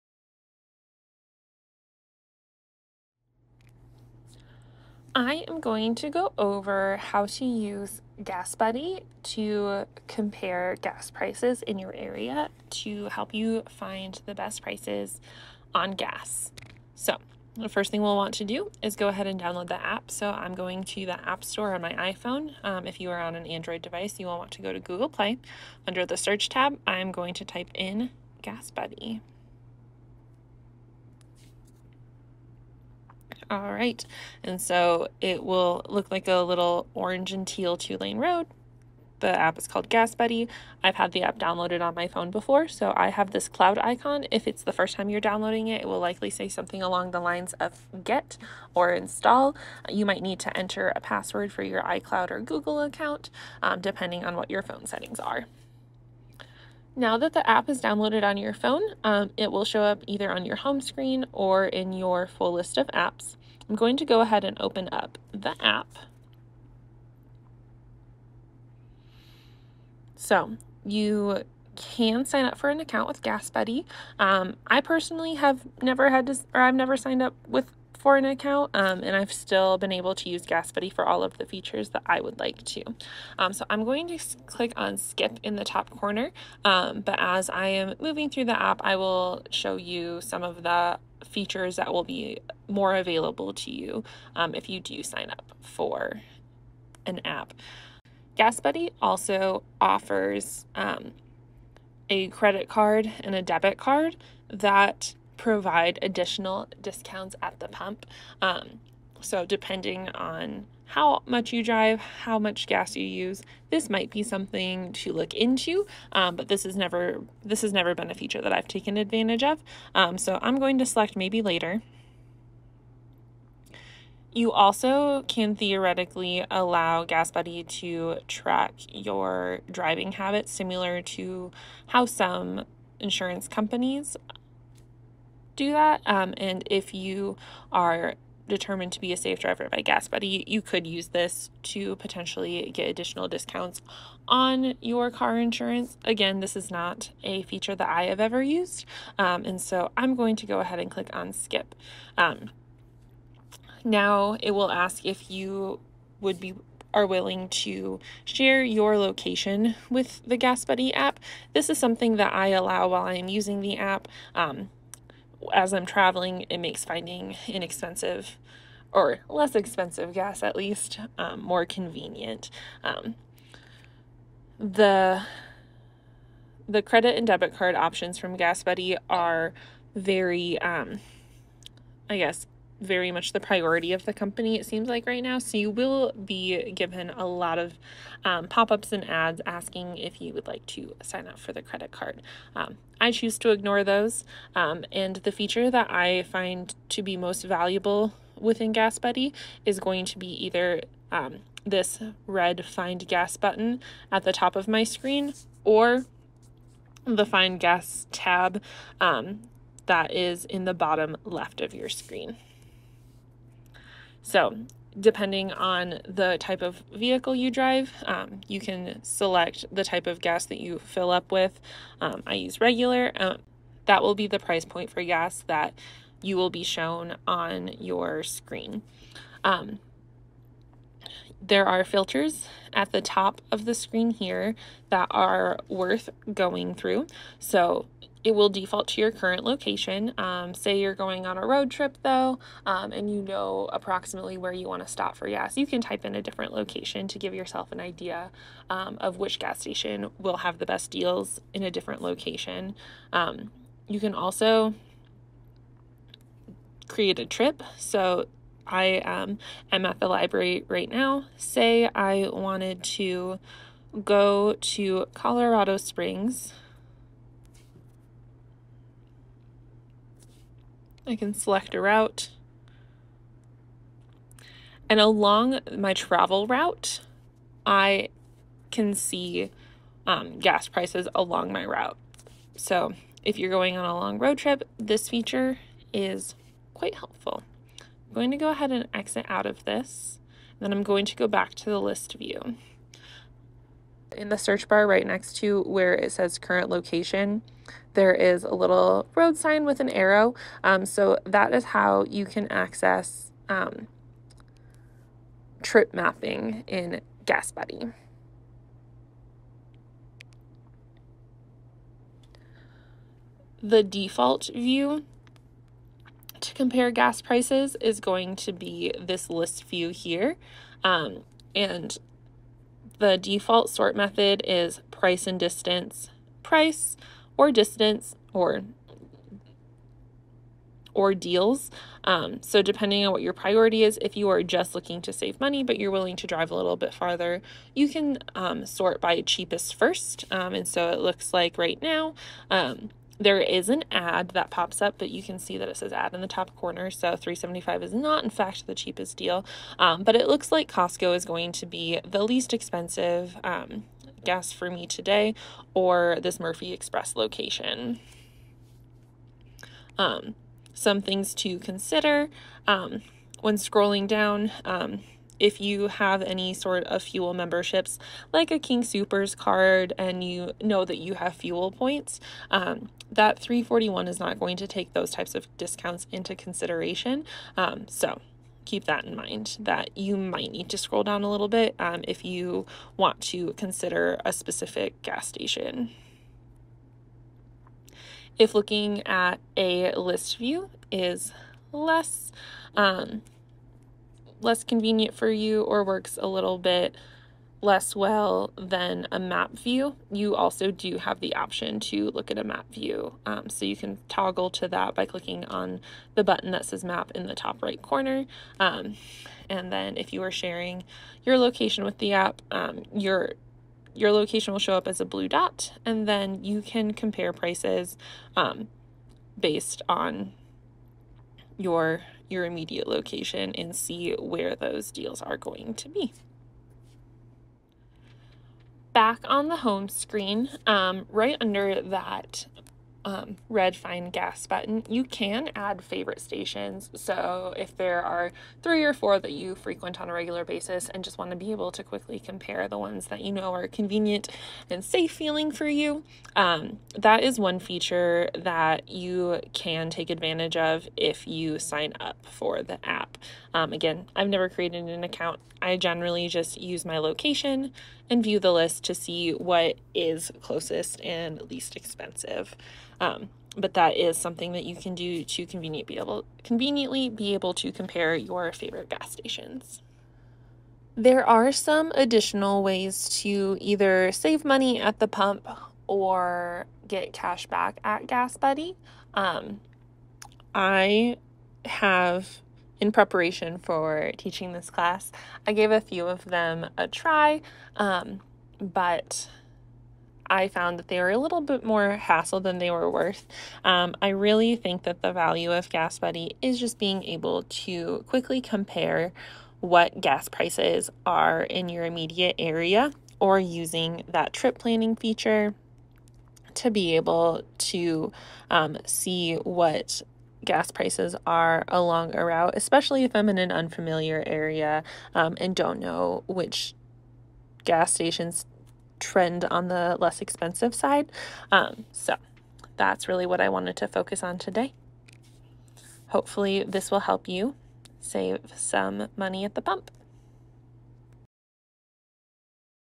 Technology Class: GasBuddy App
Shop smart with our summer series on shopping apps. Learn to better use the GasBuddy app to maximize your shopping experience.

This video was recorded in the summer of 2022, please refer for GasBuddy's most accurate and up-to-date policies.